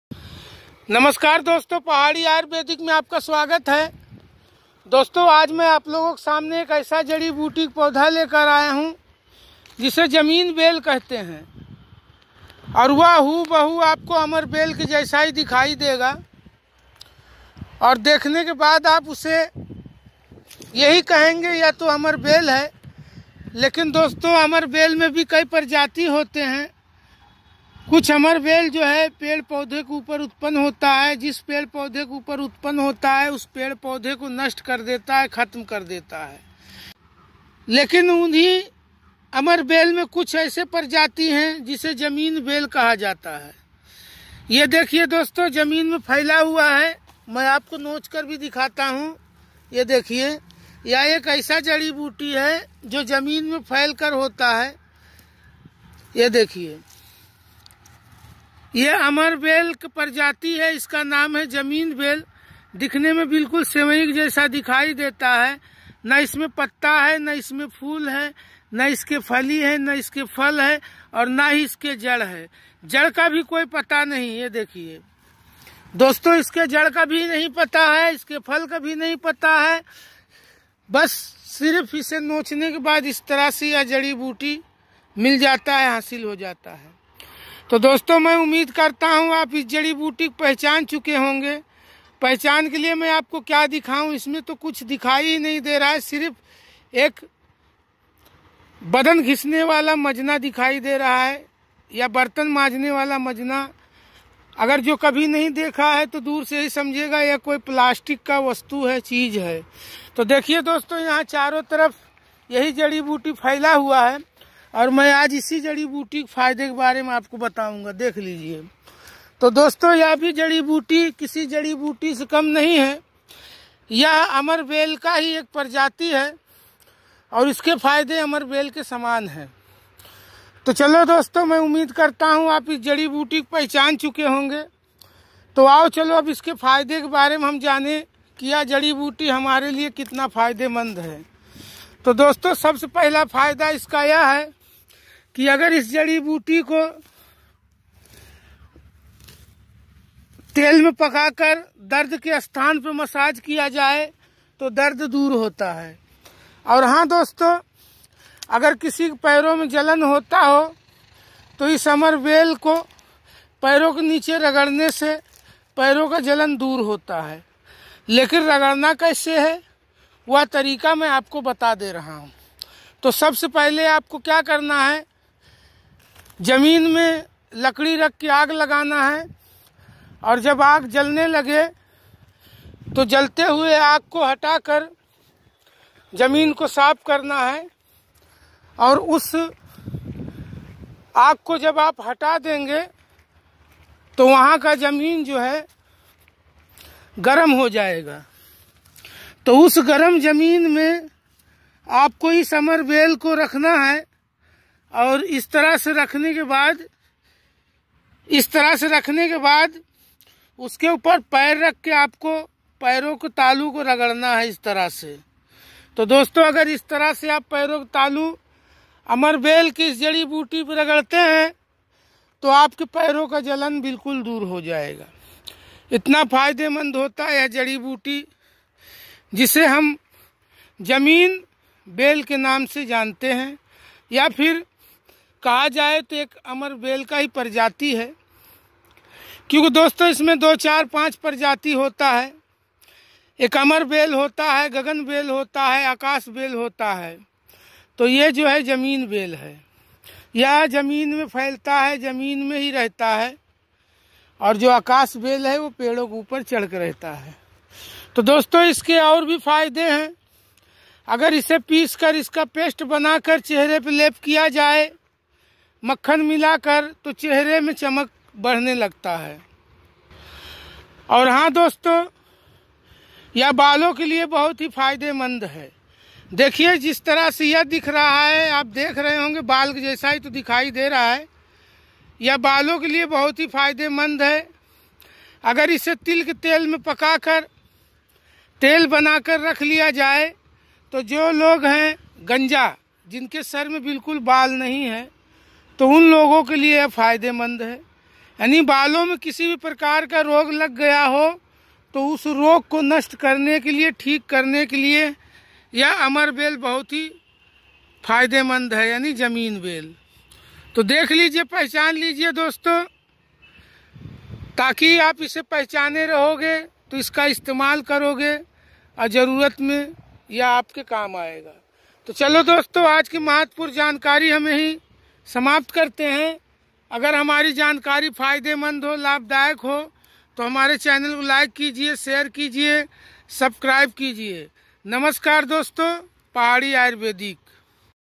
ढूढ़ते रह जाओगे इस जड़ी-बूटी को जल्दी नहीं पाओगे,अगर पा जाना छोड़ना मत बहुत चमत्कारी औषधि है यह
पहाड़ी आयुर्वेदिक के माध्यम से इस पौधे का पहचान दिखाया गया है और इसके फायदे के बारे मे बताया गया है ।